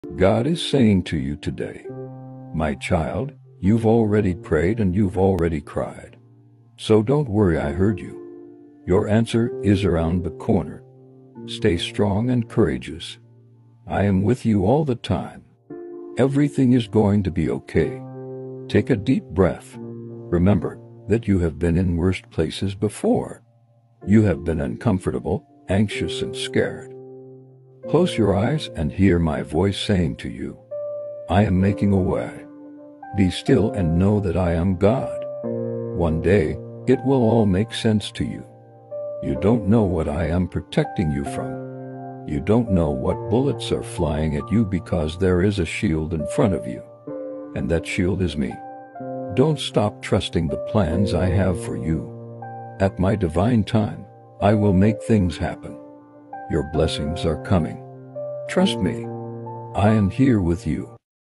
God IS SAYING TO YOU TODAY
Prayer of encourangenment. God is saying to you today, my child you've already prayed and you've already cried, so don't worry. I heard you, your answer is around the corner, stay strong and courageous. I am with you all the time everything is going to be okay, take a deep breath. You have been uncomfortable anxious and scared, close your eyes and hear my voice saying to you I'am making a way be still and know that I  am God. Amen and amen